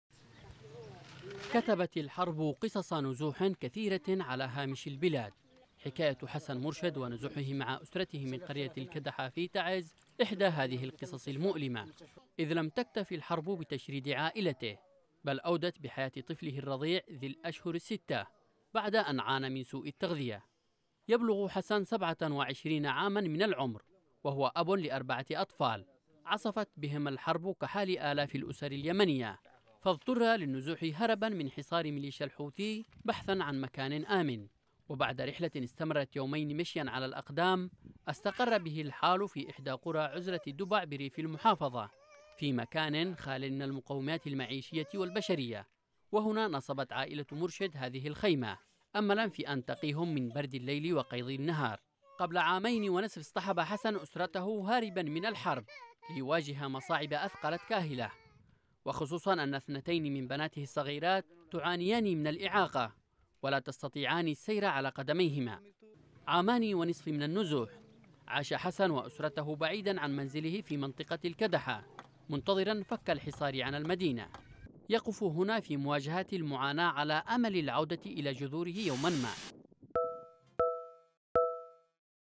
حسن مرشد.. مواطن نزح جراء الحرب في تعز ليجد نفسه وعائلته أمام فصل من المعاناة | تقرير: عمر النهمي
المواطن حسن مرشد نزح مع أسرته من قرية الكدحة بريف تعز هربا من الحرب التي تشنها مليشيا الحوثي على المنطقة، ليجد نفسه أمام معاناة جديدة بإحدى قرى عزلة دبع دون مأوى ولا خدمات أساسية سوى خيمة يحاول من خلالها أن يقي نفسه وعائلته بردَ الشتاء.
التفاصيل في هذا التقرير..